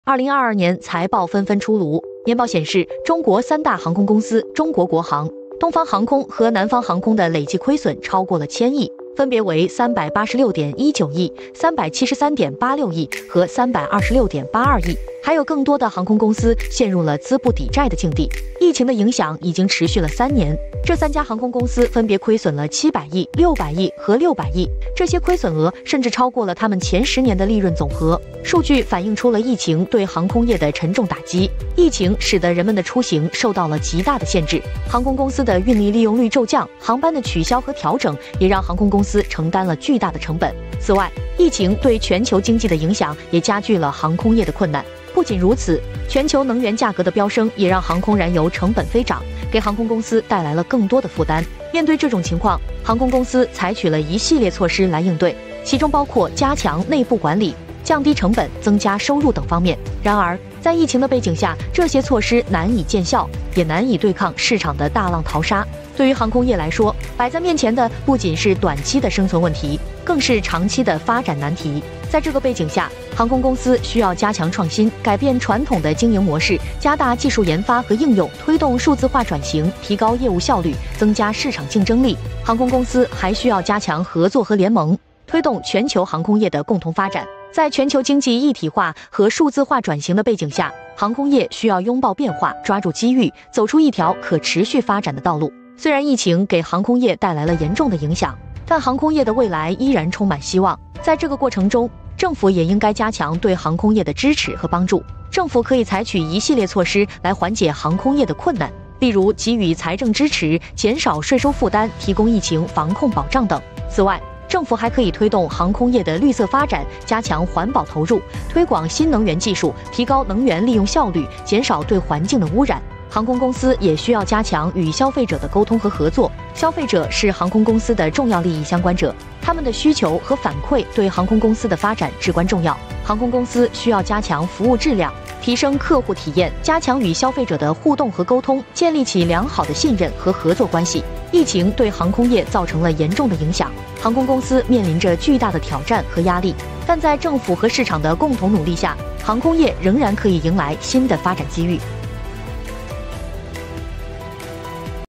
财报显示三大航空公司亏损超千亿，航空业面临挑战和压力
三大航空公司亏损超千亿，航空业面临挑战和压力